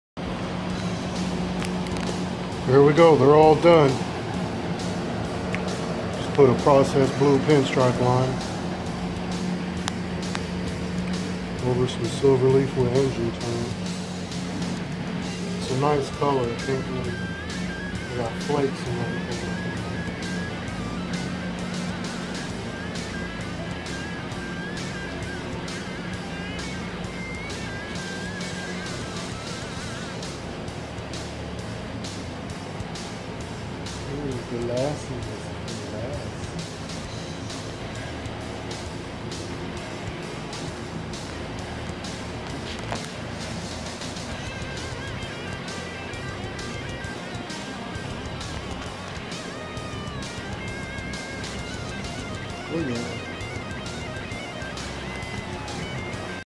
Harley-Davidson Ultra Classic full custom paint job
Blue green base mixed with 2 sizes of flake some transparent black shading. Silverleaf with engine turn in process blue pinstripe around lots of clear wet sand and buff it's beautiful these pieces here we're all the pieces that had the flowing artwork the rest of the unseen pieces were just blue green flake 26 pieces in all